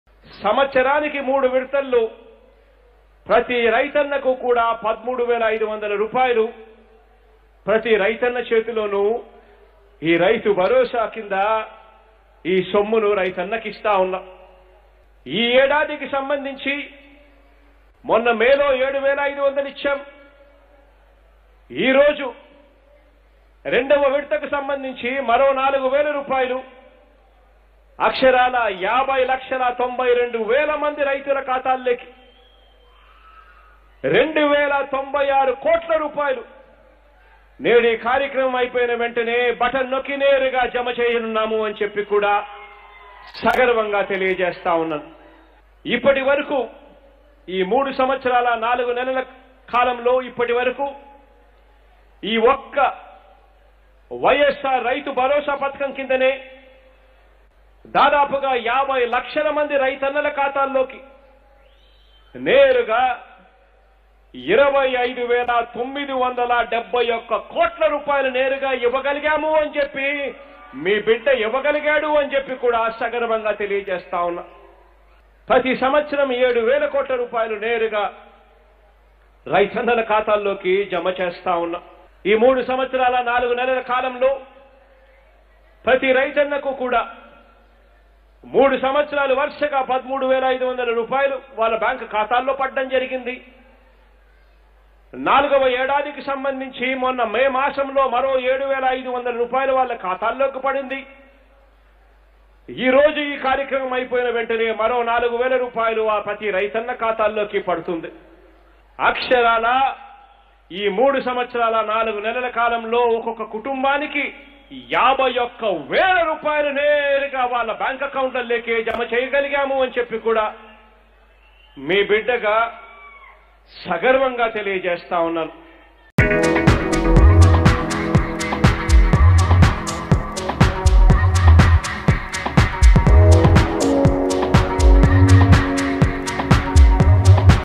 AP CM YS Jagan Releases YSR Rythu Bharosa || Public Meeting Allagadda || Sreeni News
Watch AP CM YS Jagan Releases YSR Rythu Bharosa || Public Meeting  Allagadda || Sreeni News